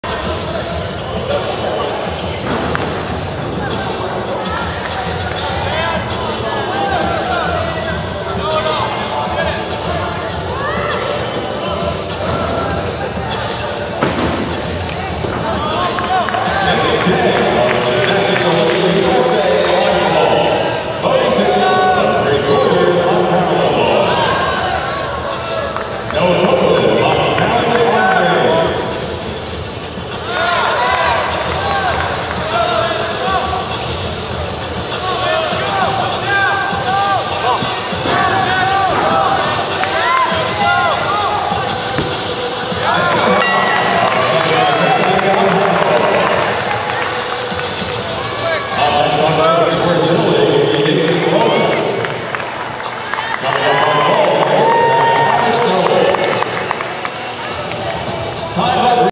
Xander Hong - High Bar - 2023 Xfinity U.S. Championships - Junior Men Day 1
Score: 12.767 (D: 4.1, E: 8.450, B: 0.217)
August 24, 2023 - SAP Center - San Jose, Cal.